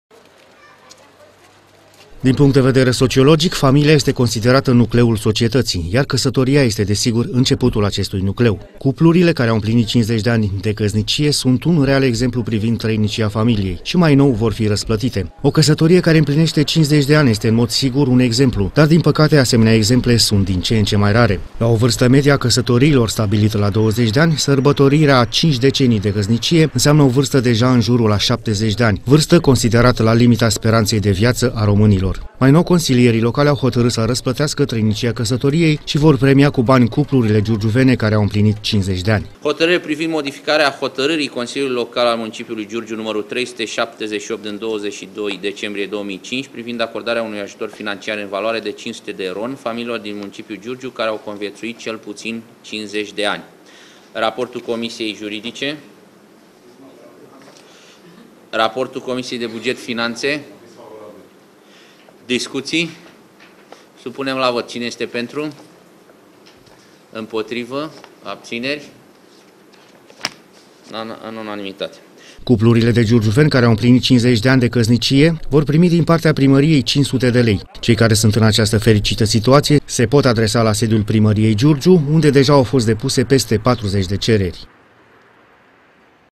50 DE ANI DE CASNICIE
Cuplurile norocoase care au depășit 50 de ani de la căsătorie vor primi din partea municipalității suma de 500 de lei. Cei care au avut șansa unei căsnicii atât de lungi pot face cerere către Primărie, atașând copii după actele de identitate, precum și după certificatul de căsătorie și după certificatele de naștere.